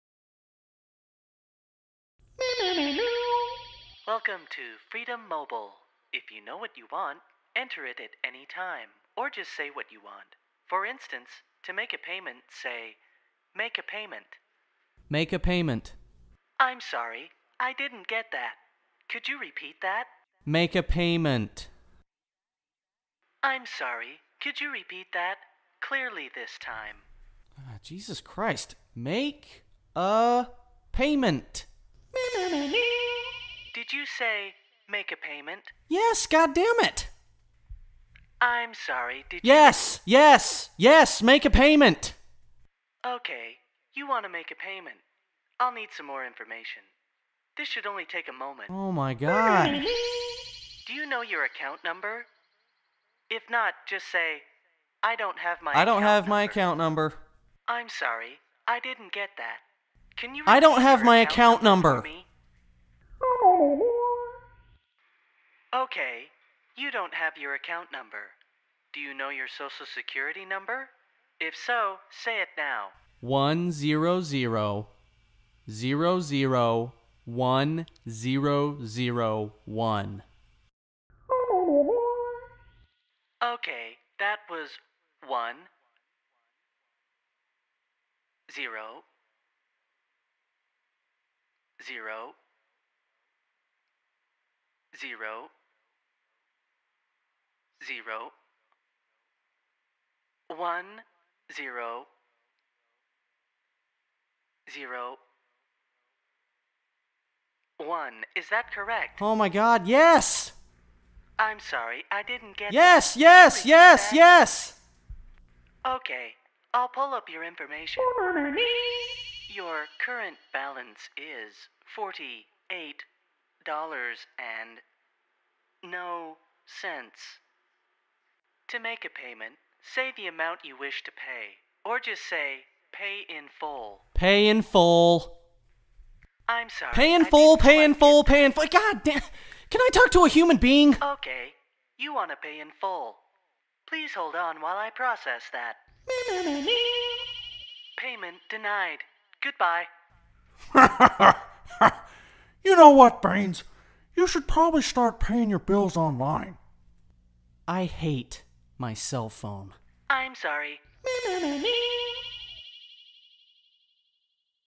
faculty&staff (short #6): i hate my cell phone
Banes tries to make a payment.